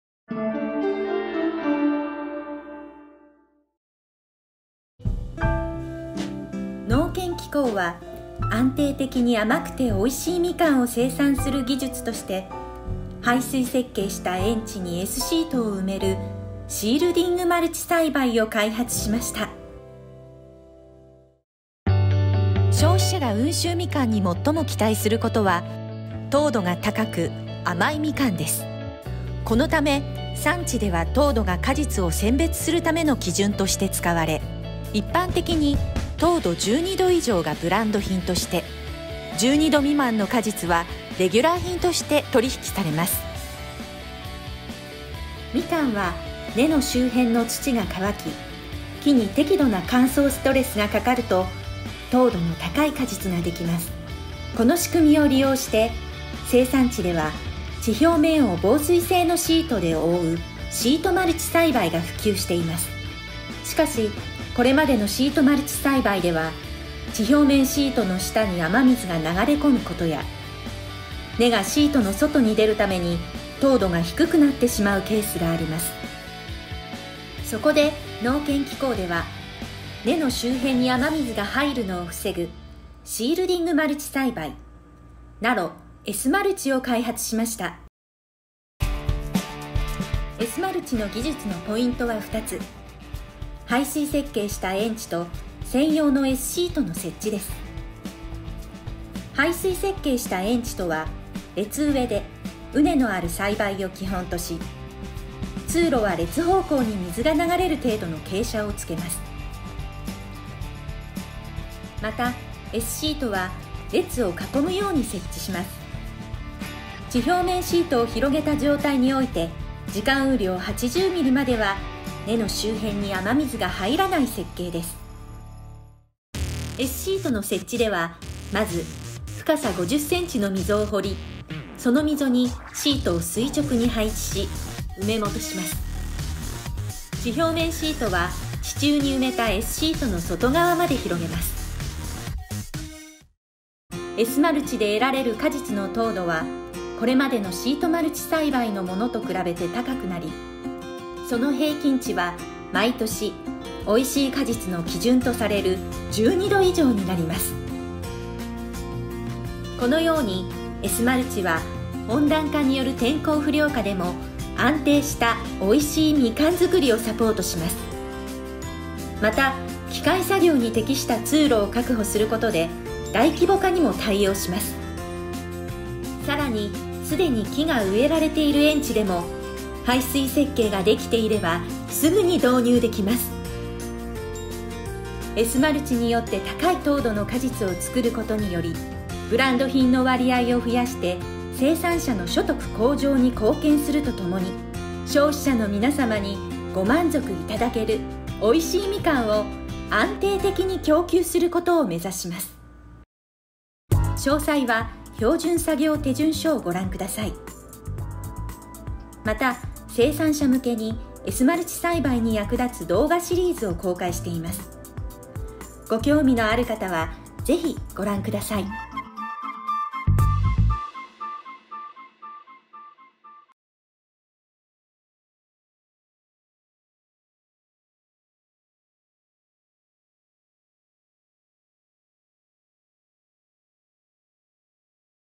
美味しいみかんを作る！ シールディング・マルチ栽培（NARO S.マルチ）
農研機構は、甘くて美味しいみかんを安定的に生産する技術として、排水設計した園地にNARO S.シートを埋めるシールディング・マルチ栽培（NARO S.マルチ）を開発しました。この技術をみかんの産地に普及させて、消費者のみなさまにご満足いただける美味しいみかんを安定的に供給することを目指します。

◆ 技術マニュアル（pdf）のご案内
S.マルチ栽培について詳しく知りたい方は技術マニュアルをご覧ください。

◆ 関連動画のご案内
生産者向けに、みかん栽培に役立つ動画を公開しています。